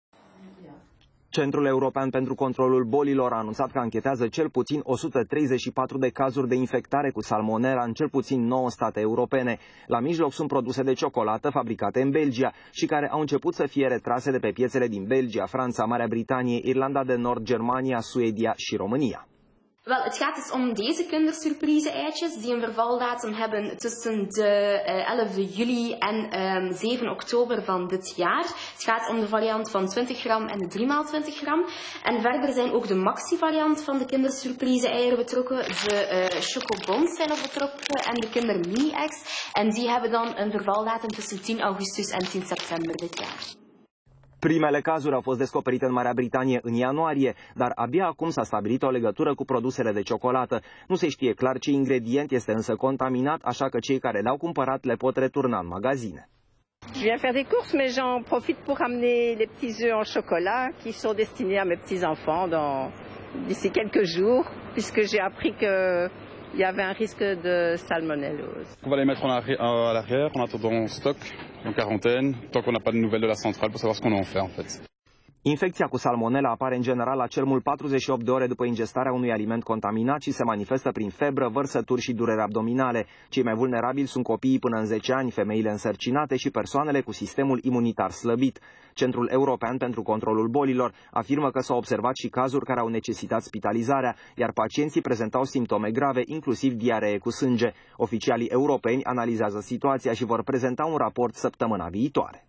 Alertă de Salmonella în Europa
Alertă de Salmonella înainte de Paşte în Europa. Iar vinovate sunt chiar ouăle de ciocolată, care au fost retrase de pe piaţă din mai multe state, inclusiv din România. Cei mai afectaţi sunt copiii sub 10 ani, care pe alocuri prezintă simptome severe şi necesită spitalizare.